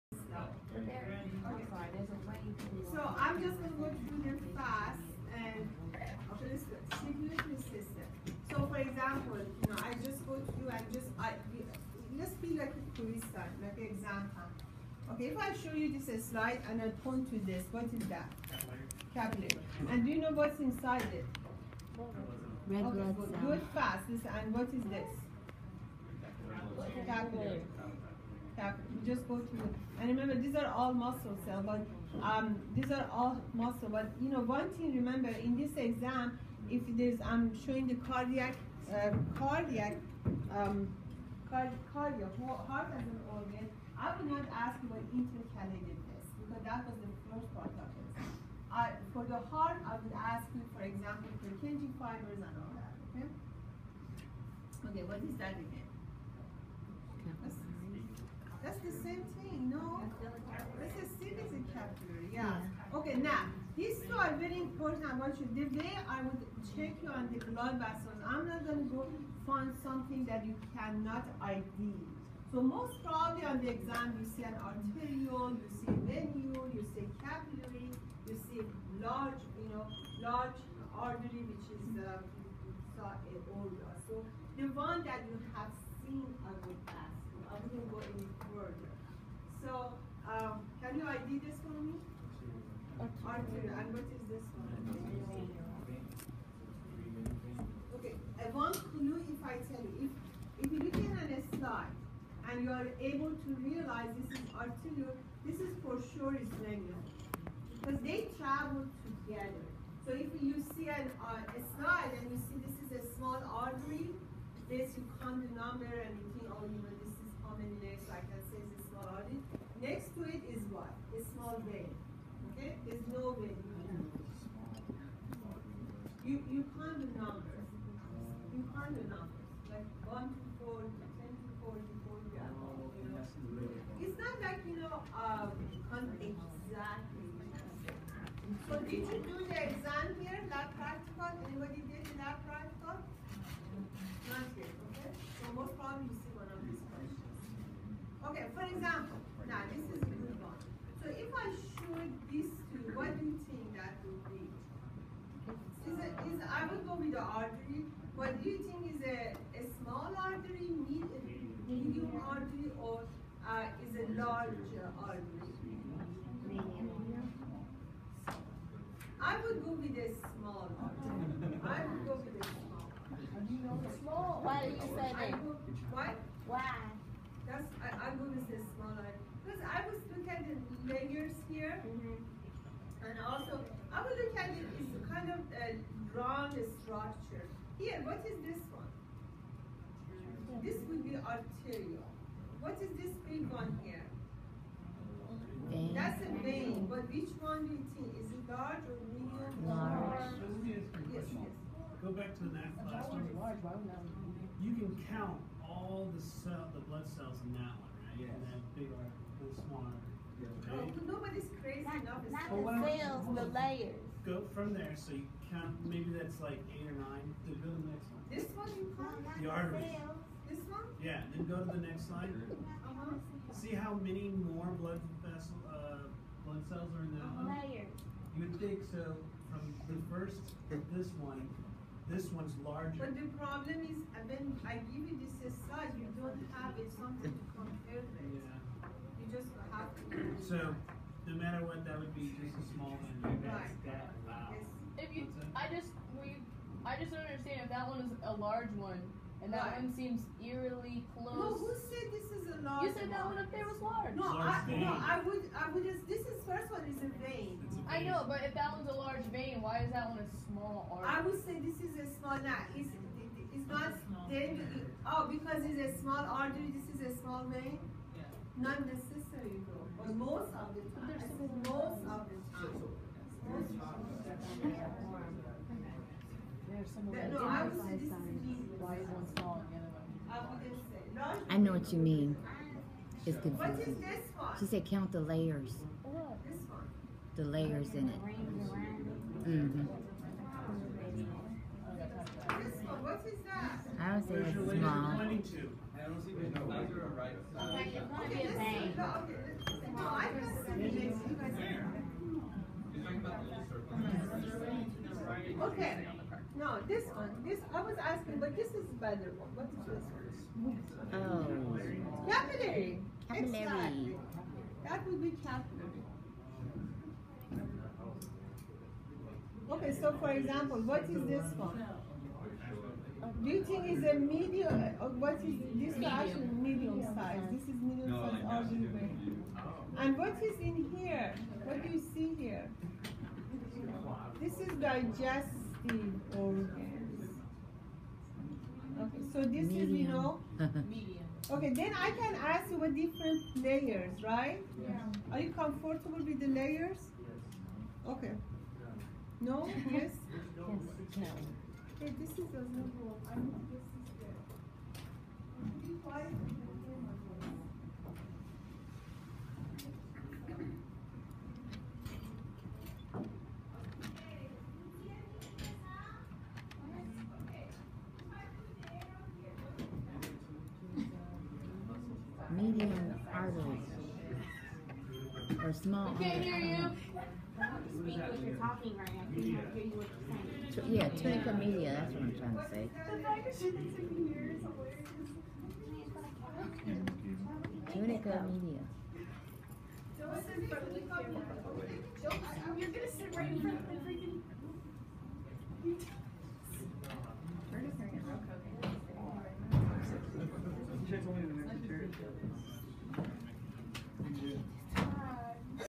C&T Practical 2 - Review Part 1
This Part 1 video (6:45) was recorded with an extremely low volume. Part 2 is a continuation for 58 minutes, which is inclusive of the entire Practical 2 review for Dr. Sokhansanj at Parker University and is located on this channel.